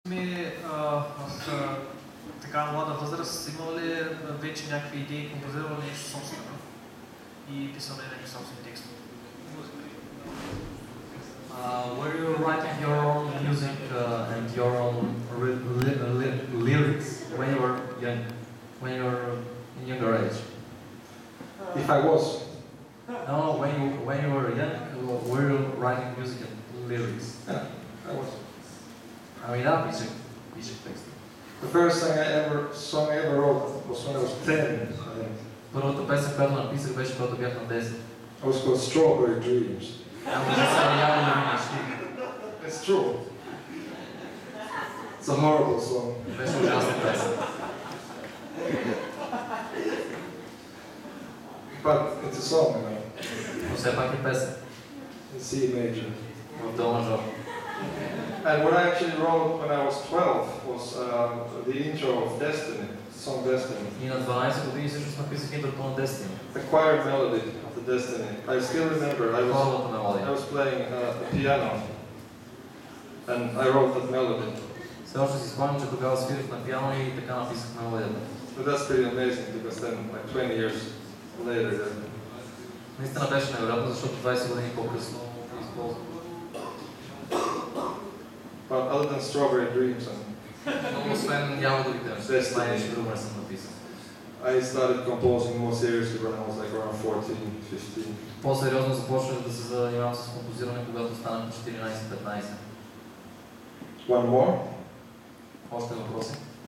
Timo Tolkki - Masterclass Seminar Europe 2010 - Music Beyond Infinity @ Club R.B.F.
Timo Tolkki - Masterclass Seminar Europe 2010 - Music Beyond Infinity @ Club R.B.F. - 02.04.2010 - Sofia, Bulgaria